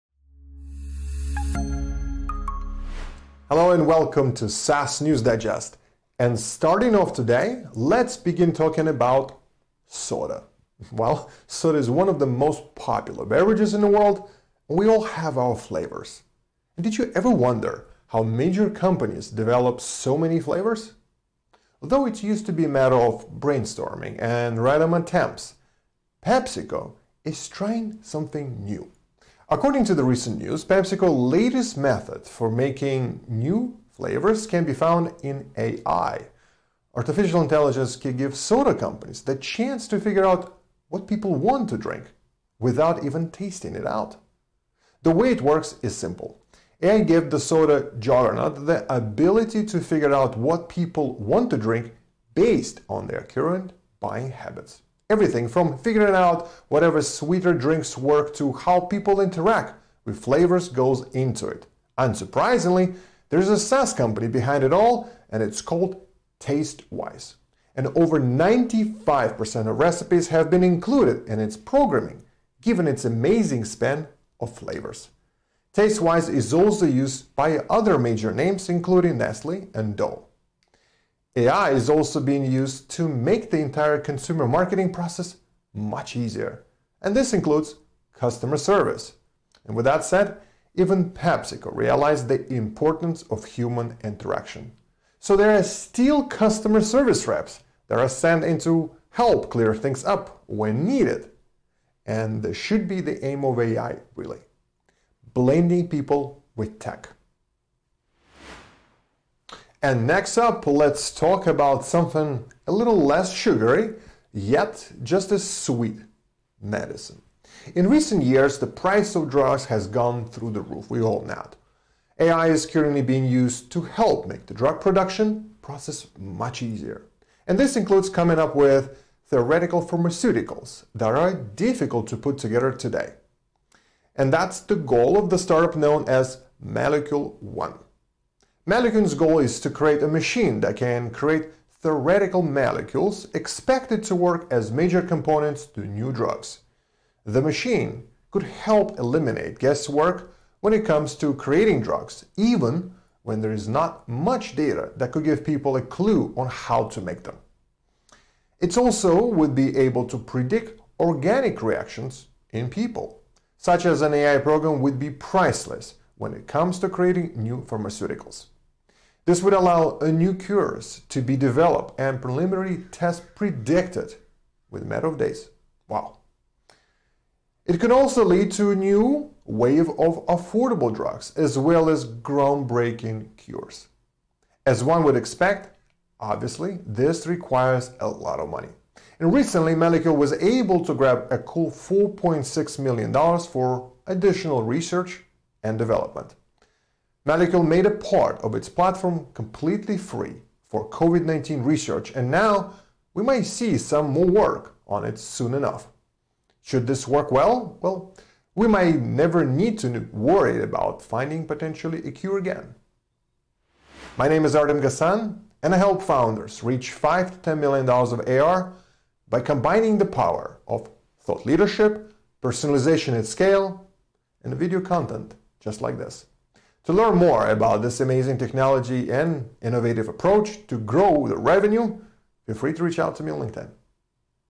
PepsiCo Uses AI For New Flavors - SaaS News Digest #106
✅ PepsiCo Uses AI For New Flavors
✅ The Company Making New Drugs Via AI
👉 Watch Secrets of Rapid Revenue and Growth on SaaS Talk Show